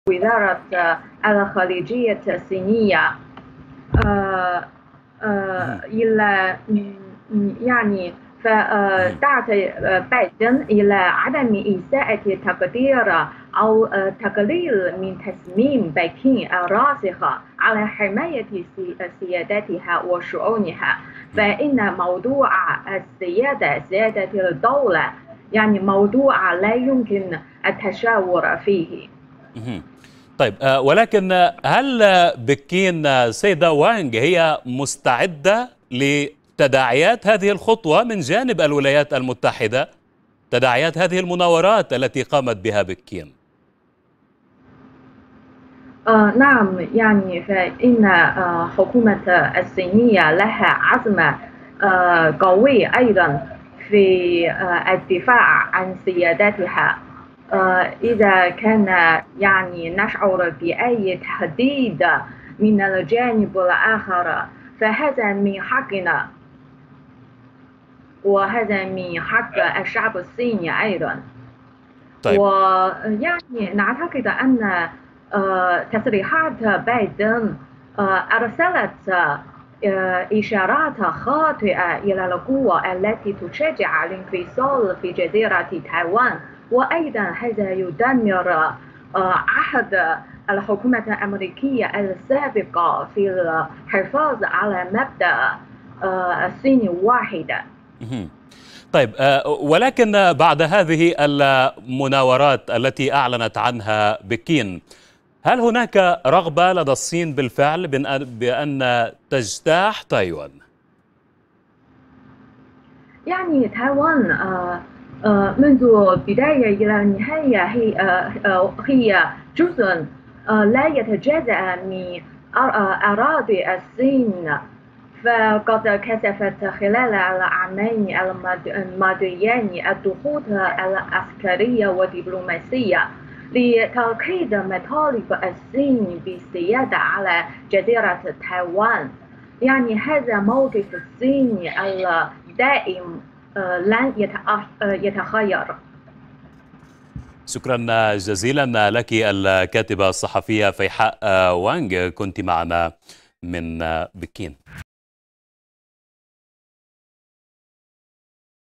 صحفية تكشف سر توقيت تحذير الصين لأمريكا بتدريبات عسكرية حول تايوان
وبينما تدور الحرب في أوكرانيا ، تسود حالة من الترقب، بشأن الموقف الأمريكي من تايوان حيث، أعلنت الصين إجراء تدريبات "عسكرية" قرب تايوان،

"كتحذير جدي" للولايات المتحدة، من عاقبة تفاعلها مع تايوان، باعتبارها جزءا من أراضيها، بموجب سياسة "الصين الواحدة".

المزيد من التفاصيل تتحدث من بكين الكاتبة الصحفية فيحاء وانغ مع الإعلامي حسين حسني